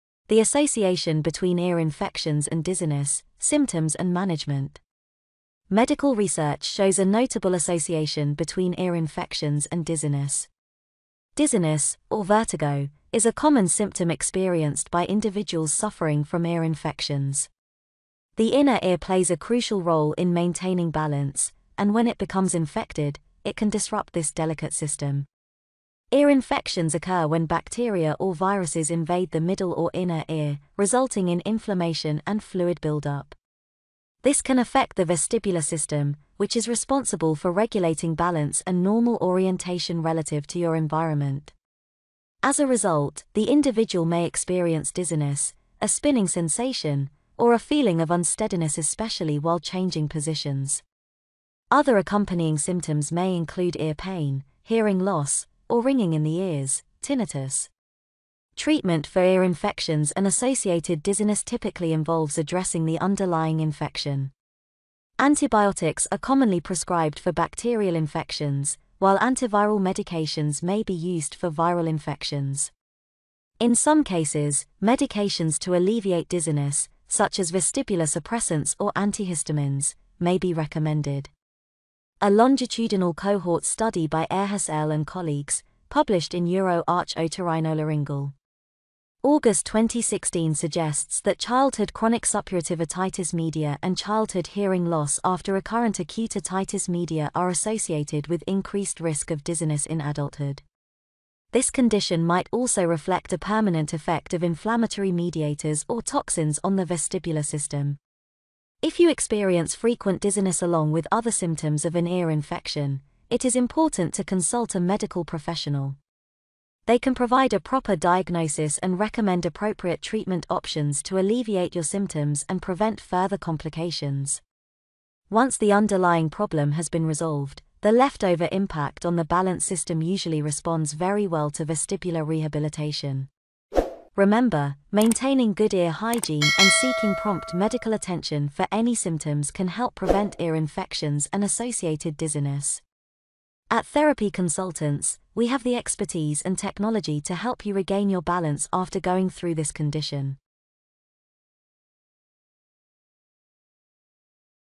The Association Between Ear Infections and Dizziness: Symptoms and Management.
Medical research shows a notable association between ear infections and dizziness. Dizziness, or vertigo, is a common symptom experienced by individuals suffering from ear infections. The inner ear plays a crucial role in maintaining balance, and when it becomes infected, it can disrupt this delicate system.

Ear infections occur when bacteria or viruses invade the middle or inner ear, resulting in inflammation and fluid buildup. This can affect the vestibular system, which is responsible for regulating balance and normal orientation relative to your environment.

As a result, the individual may experience dizziness, a spinning sensation, or a feeling of unsteadiness especially while changing positions. Other accompanying symptoms may include ear pain, hearing loss, or ringing in the ears (tinnitus).

Treatment for ear infections and associated dizziness typically involves addressing the underlying infection. Antibiotics are commonly prescribed for bacterial infections, while antiviral medications may be used for viral infections. In some cases, medications to alleviate dizziness, such as vestibular suppressants or antihistamines, may be recommended.

A longitudinal cohort study by Aarhus L and colleagues, published in Eur Arch Otorhinolaryngol. August 2016 suggests that childhood chronic suppurative otitis media and childhood hearing loss after recurrent acute otitis media are associated with increased risk of dizziness in adulthood. This condition might also reflect a permanent effect of inflammatory mediators or toxins on the vestibular system.

If you experience frequent dizziness along with other symptoms of an ear infection, it is important to consult a medical professional. They can provide a proper diagnosis and recommend appropriate treatment options to alleviate your symptoms and prevent further complications. Once the underlying problem has been resolved, the left-over impact on the balance system usually responds very well to vestibular rehabilitation.

Remember, maintaining good ear hygiene and seeking prompt medical attention for any symptoms can help prevent ear infections and associated dizziness.

At Therapy Consultants, we have the expertise and technology to help you regain your balance after going through this condition.